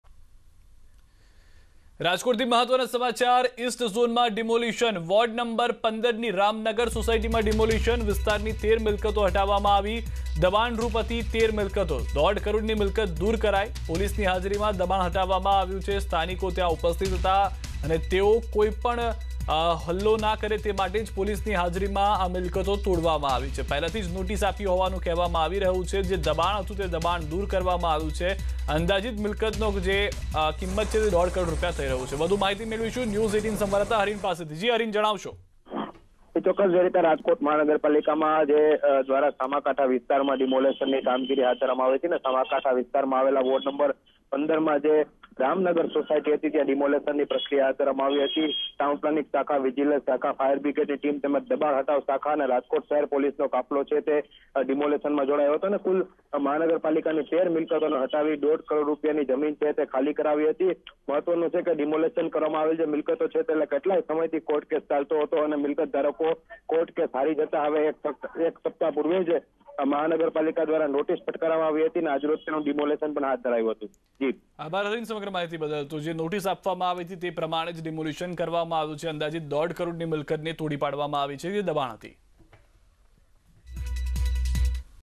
Rajkot: ઇસ્ટઝોનમાં હાથ ધરાયું ડિમોલિશન
Rajkotના ઇસ્ટ ઝોનમાં આવેલી રામનગર સોસાયટીમાં ડિમોલિશનની કામગીરી હાથ ધરવામાં આવી છે. આ વિસ્તારમાં દબાણ કરતી કુલ 13 મિલ્કતો દૂર કરવામાં આવી છે જેની કિંમત આશરે દોઢ કરોડ આંકવામાં આવી હતી. પોલીસની હાજરીમાં આ કાર્યવાહી કરવામાં આવી હતી.